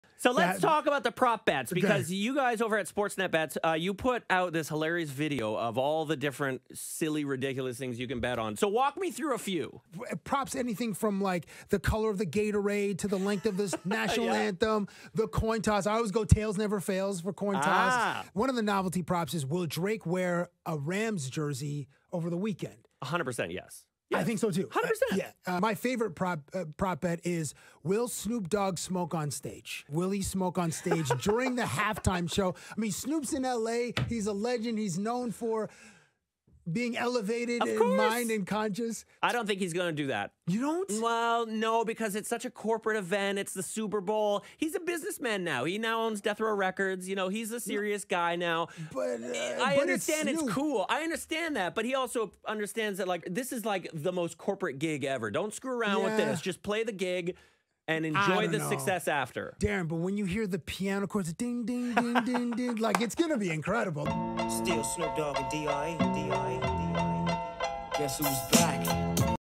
WILL SNOOP LIGHT UP ON STAGE AT THE SUPERBOWL??
And other hilarious prop bets with Saryn Jones and Cabbie